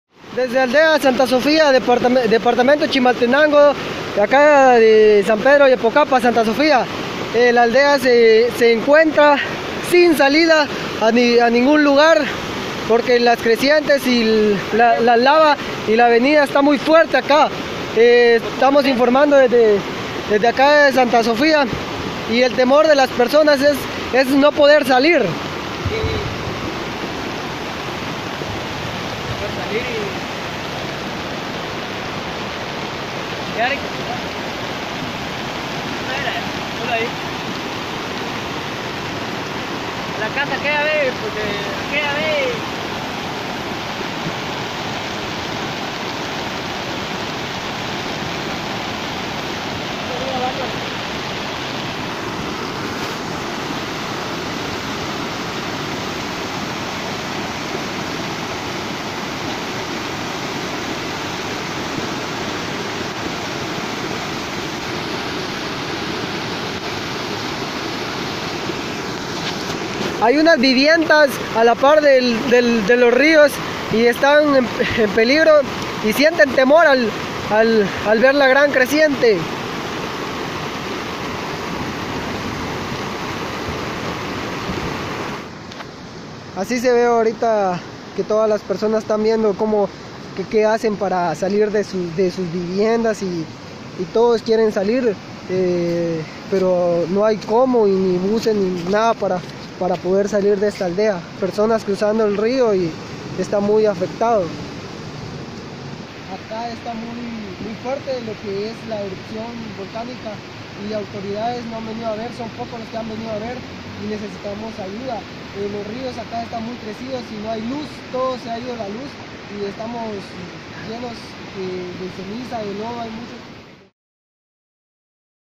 Descienden tres lahares fuertes en barrancas del Volcán de Fuego | Prensa Libre
GUATEMALA. El Instituto Nacional de Sismología, Vulcanología, Meteorología e Hidrología (Insivumeh) advirtió en un boletín de que se presentaron los primeros deslizamientos de material volcánico y rocas, por lo que no recomienda la presencia de personas en las zonas de impacto. (Video Prensa Libre: Imágenes cortesía Levin Sánchez)

Lea en PrensaLibre.com: Descienden tres lahares fuertes en barrancas del Volcán de Fuego

Lo invitamos a ver más contenido en
Nuestra web: